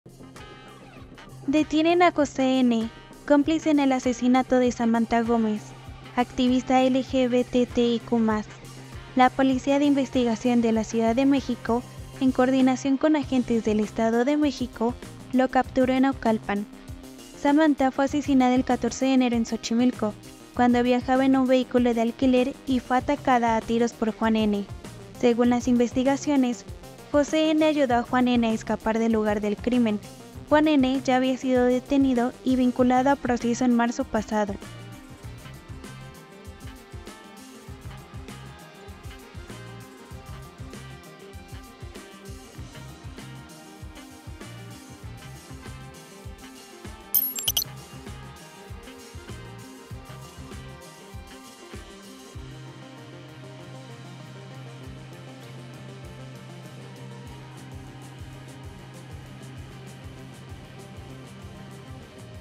Detienen a cómplice de asesino de Samantha Gómez, activista LGBTTTIQ+
El agresor sería un hombre llamado Juan "N", quien escapó en un vehículo que posiblemente conducía José “N”.